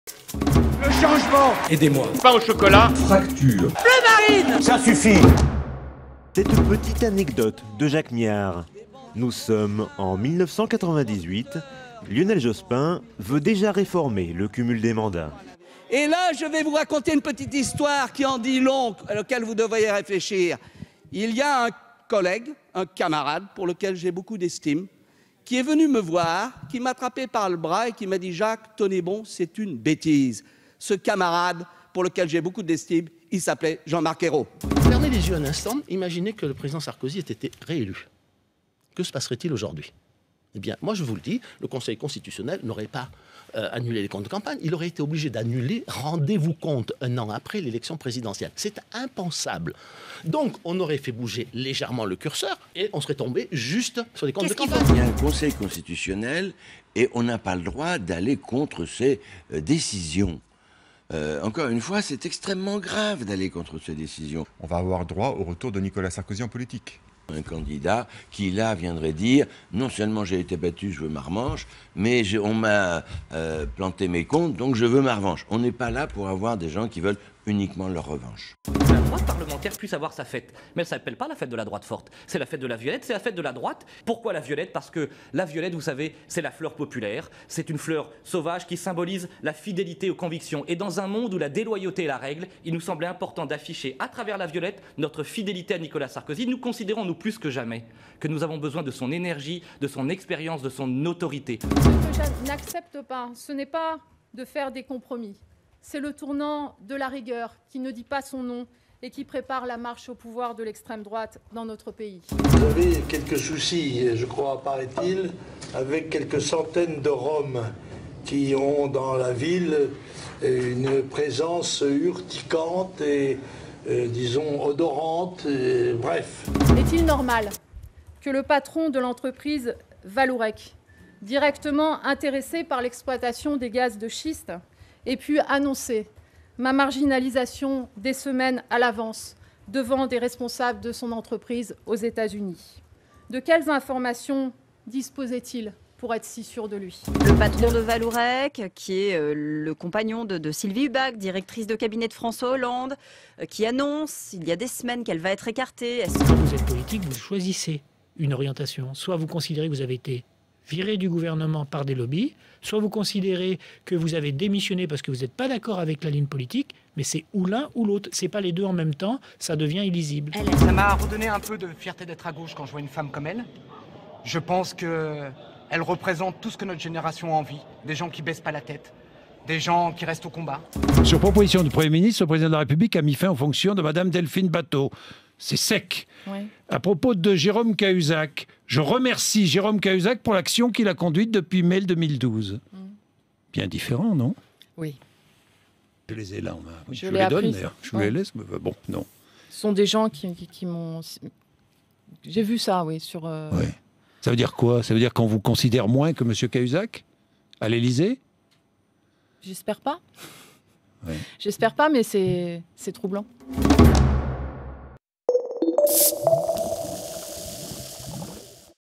Cumul des mandats : l'anecdote de Myard qui détruit Ayrault
Retrouvez chaque jour à midi les déclarations politiques de la matinée qu'il ne fallait pas manquer.